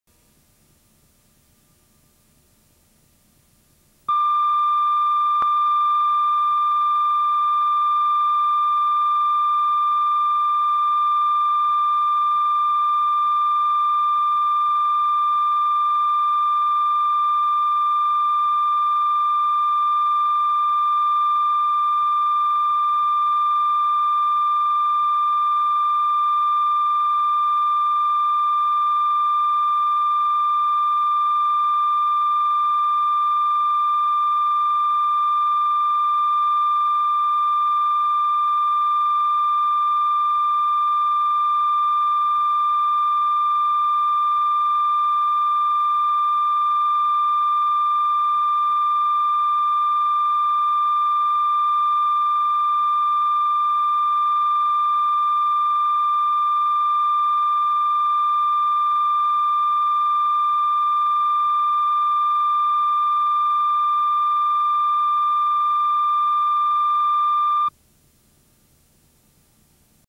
White screen test pattern (1167 Hz tone) (test pattern ONLY version) [True HQ] [1080p60]
Since nobody has a copy of this test pattern from a Fotomat VHS/Betamax uploaded here currently, I uploaded it. From the 1979 Fotomat VHS of "The Godfather".
Trivia: This test pattern would later appear again on the 1982 MCA Videocassette Inc. VHS/Betamax of "The Amazing Spider-Man".
This is a test pattern only version that's great for looping. See the full version here, too: